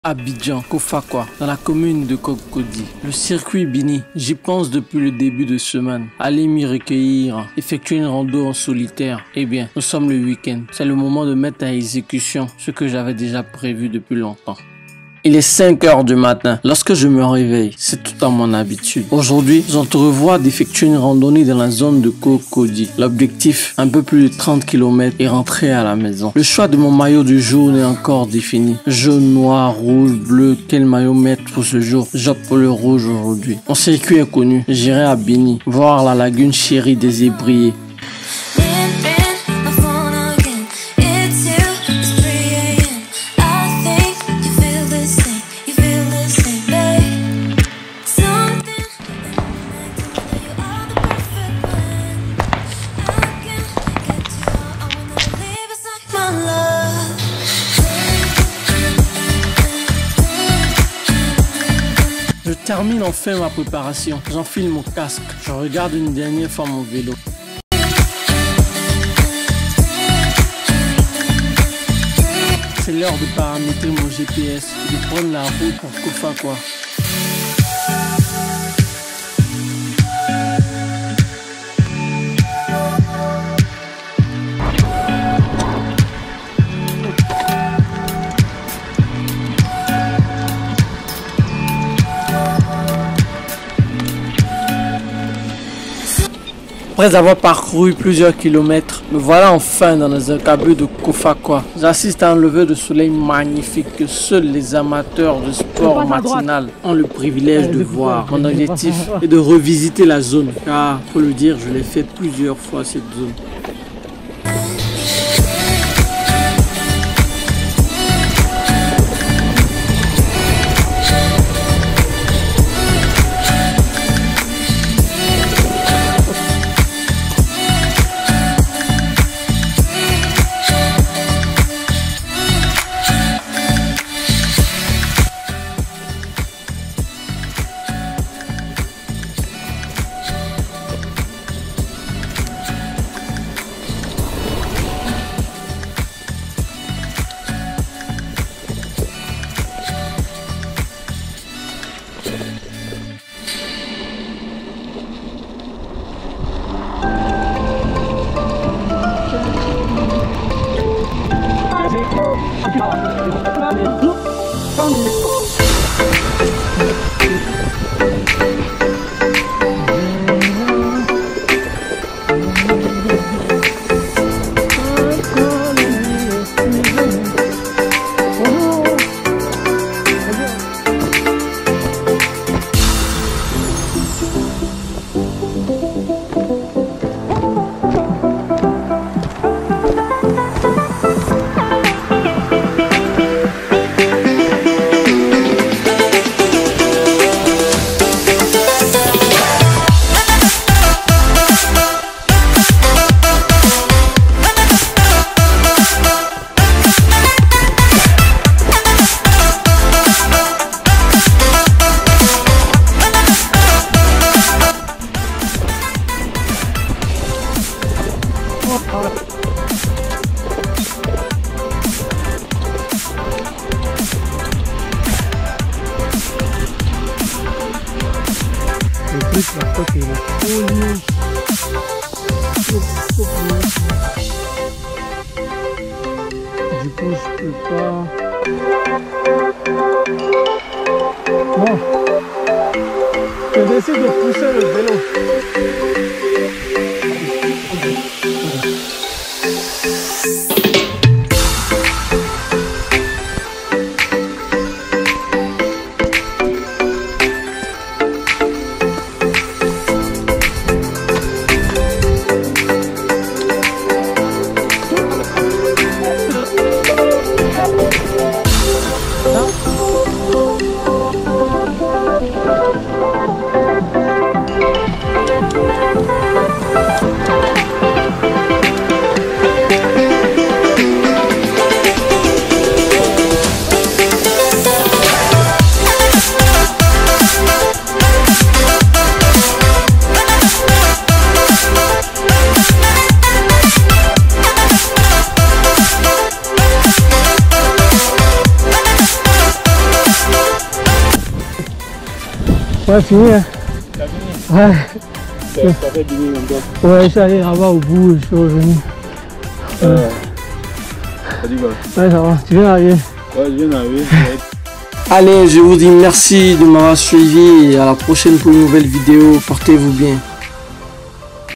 KOFFAKOI ( ALLONS À BINI LAGUNE )
Hey les amis , je reviens avec une nouvelle vidéo sur le circuit mythique de koffakoi.
La majeur partie des cyclistes de la capitale ont déjà fait du vélo dans cette zone.
J'ai pris plaisir pendant cette sortie, j'ai respirer le grand air pur et j'espère vraiment que cet endroit ne vas pas disparaitre de si tôt vu les sociétés immobilières qui sont déjà présente dans la zone.